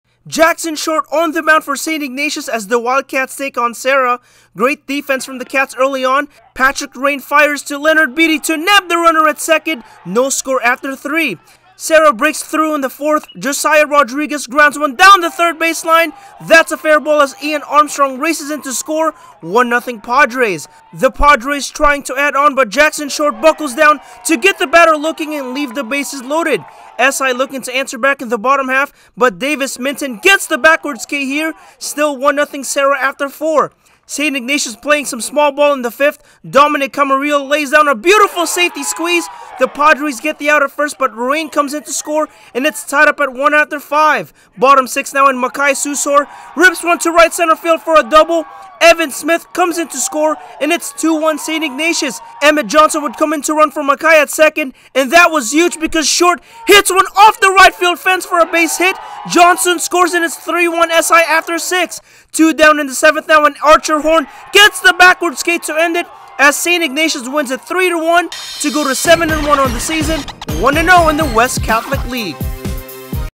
SERRA VS ST. IGNATIUS BASEBALL 3.16.23
JACKSON SHORT ON THE MOUND FOR ST. IGNATIUS AS THE WILDCATS TAKE ON SERRA

GREAT DEFENSE FROM THE CATS EARLY ON…PATRICK RUANE FIRES TO LEONARD BEATIE TO NAB THE RUNNER AT SECOND…NO SCORE AFTER 3.

SERRA BREAK THROUGH IN THE FOURTH…JOSIAH RODRIGUEZ GROUNDS ONE DOWN THE THIRD BASE LINE…THAT’S A FAIR BALL AS IAN ARMSTRONG RACES IN TO SCORE FROM SECOND…1-0 PADRES

THE PADRES TRYING TO ADD ON BUT JACKSON SHORT BUCKLES DOWN TO GET THE BATTER LOOKING AND LEAVE THE BASES LOADED.

SI LOOKING TO ANSWER BACK IN THE BOTTOM HALF BUT DAVIS MINTON GETS THE BACKWARDS K HERE…STILL 1-0 SERRA AFTER 4.

ST. IGNATIUS WITH SOME SMALL BALL IN THE FIFTH…DOMINIC CAMARILLO LAYS DOWN A BEAUTIFUL SAFETY SQUEEZE…THE PADRES GET THE OUT AT FIRST BUT RUANE COMES IN TO SCORE AND IT’S TIED UP AT 1 AFTER 5.

BOTTOM 6 NOW AND MAKAI SUSOR RIPS ONE TO RIGHT CENTER FIELD FOR A DOUBLE…EVANN SMITH SCORES AND IT’S 2-1 ST. IGNATIUS

EMMETT JOHNSON WOULD COME IN TO RUN FOR MAKAI AT SECOND AND THAT WOULD BE HUGE BECAUSE JACKSON SHORT LINES ONE TO RIGHT FOR A BASE HIT…JOHNSON COMES IN TO SCORE AND IT’S 3-1 SI AFTER 6

TWO DOWN IN THE SEVENTH NOW AND ARCHER HORN GETS THE BACKWARDS K TO END IT AS ST. IGNATIUS WINS IT 3-1 TO GO TO 7-1 ON THE SEASON…1-0 IN THE WEST CATHOLIC LEAGUE